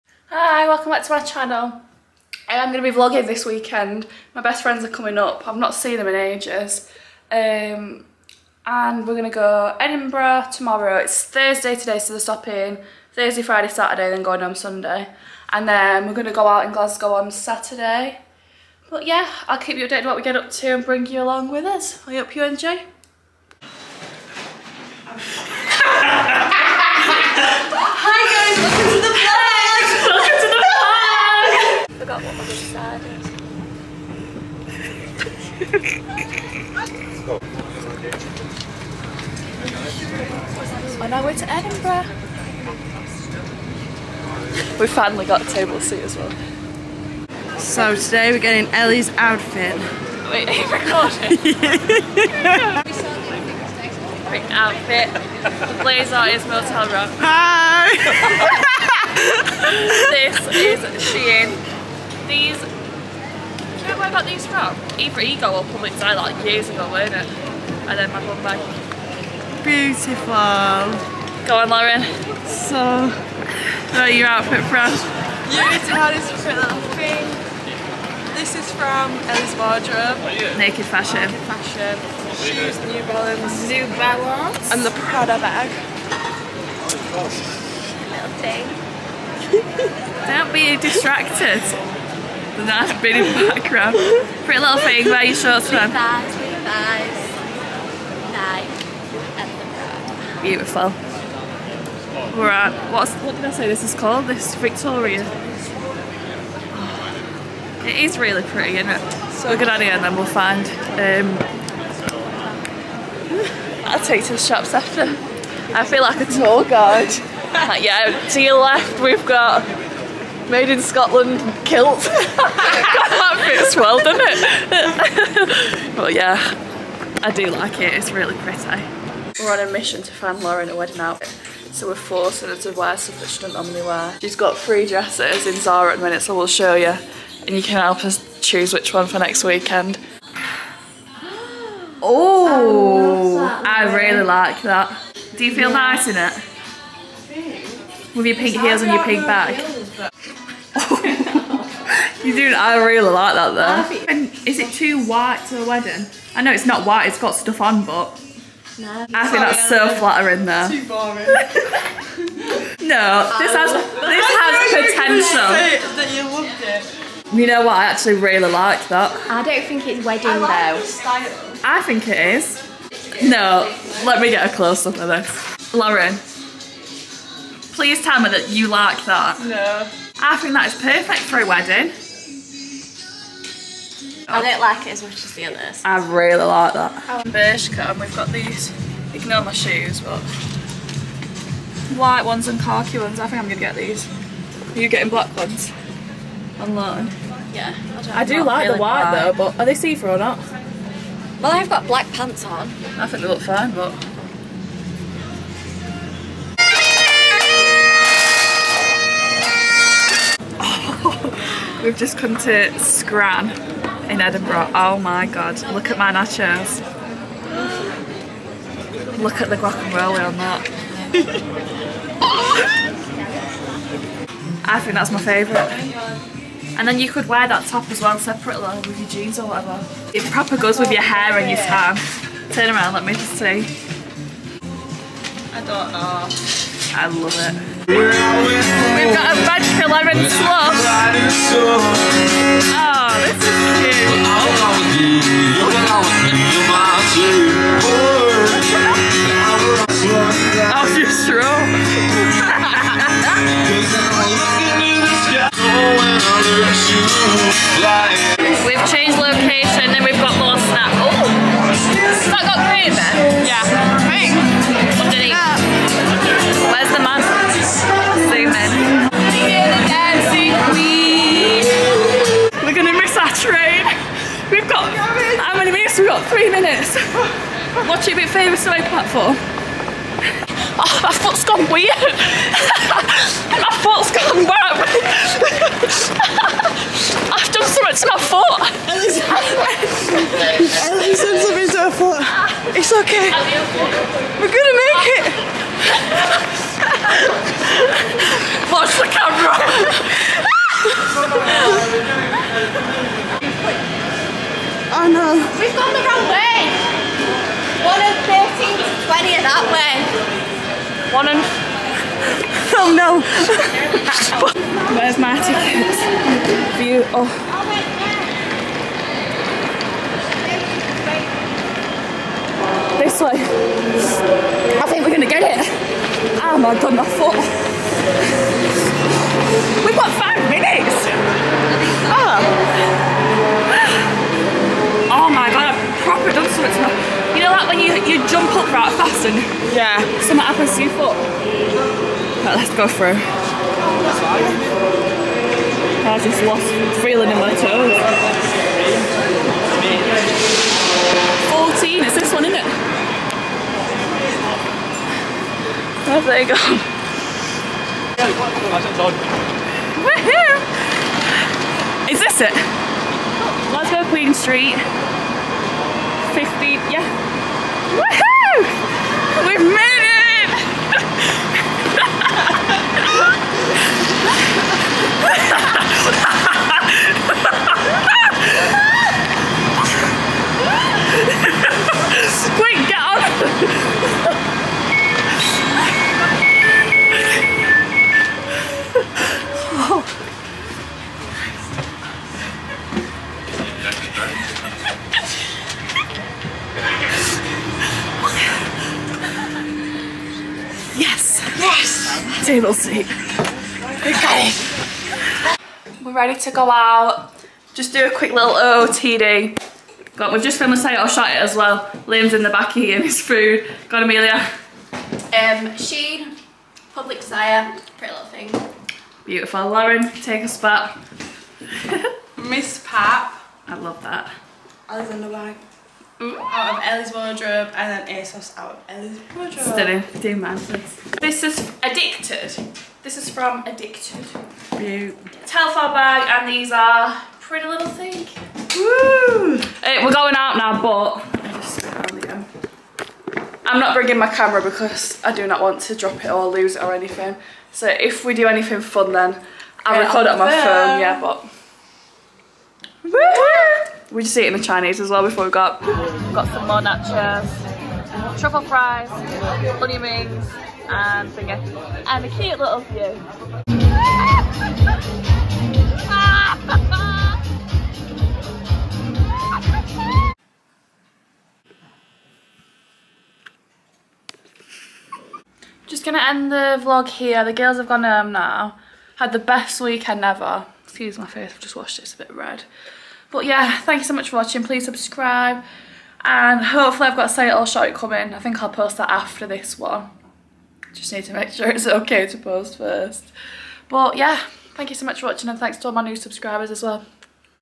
GIRLS WEEKEND VLOG!! | shopping in Edinburgh and Glasgow night out
Lauren’s Instagram: @laurennclegg
Amelia’s: @ameliagshaw